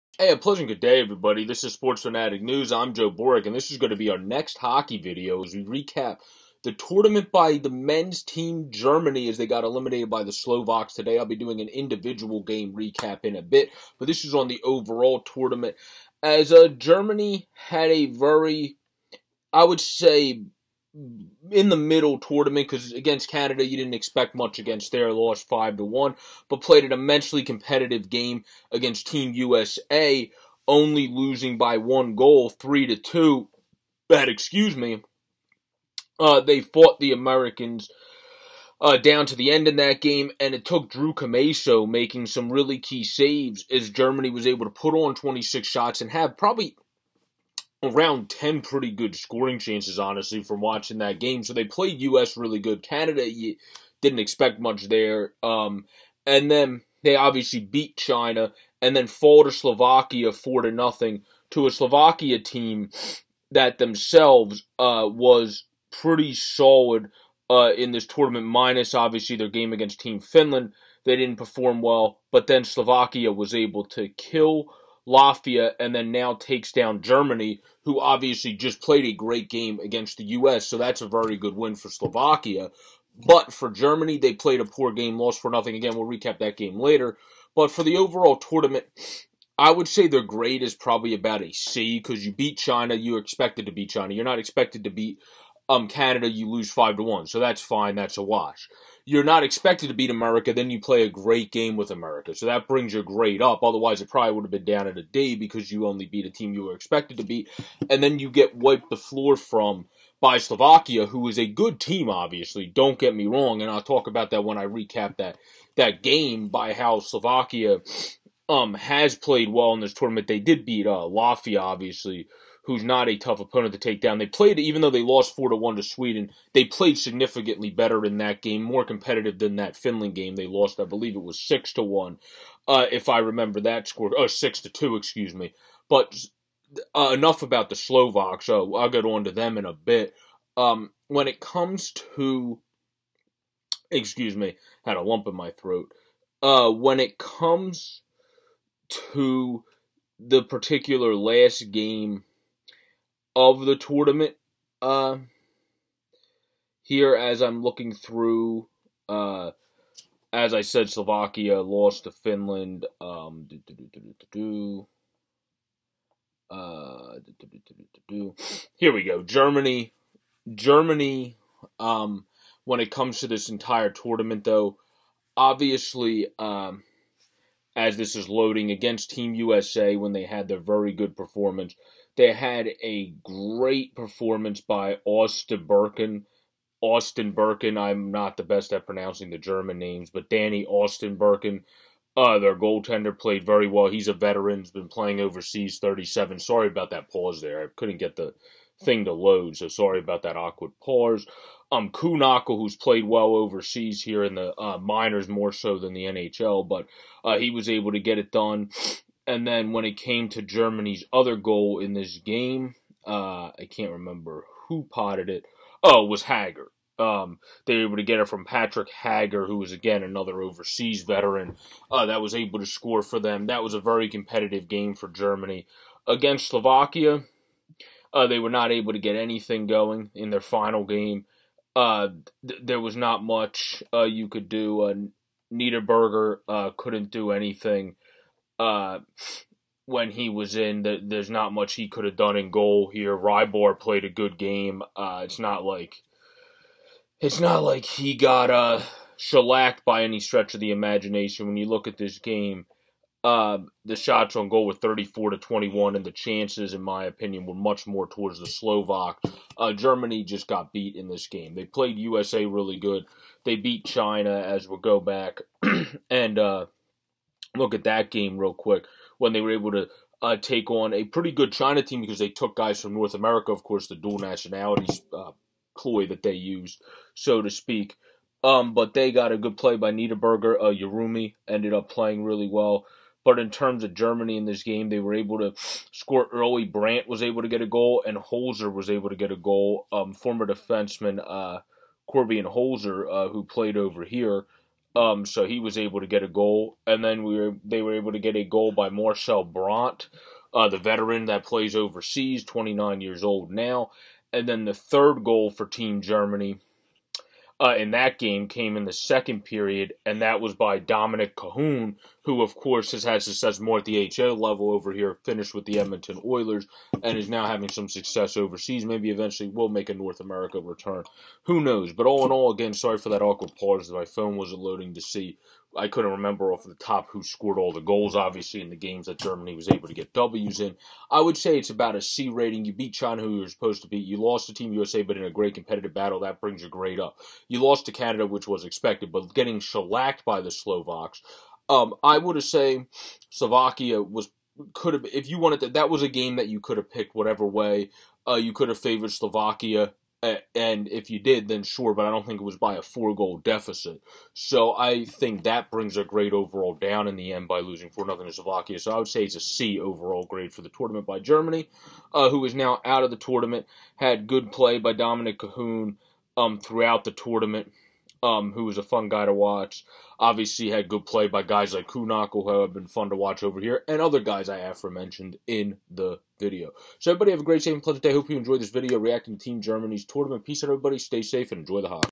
Reacting to Team Germany Men's Hockey Play in the Olympics after Elimination
In This one Pro Joe Gets to React to a team who played below expectations to some and about on par to others in this Reacting to Team Germany Men's Hockey Play in the Olympics after Elimination video. The Battled US hard and defeated China like they should have but otherwise... Check out the video to hear the grade Pro Joe gave them for the tournament plus more on their overall performance. Thanks all and have a great day.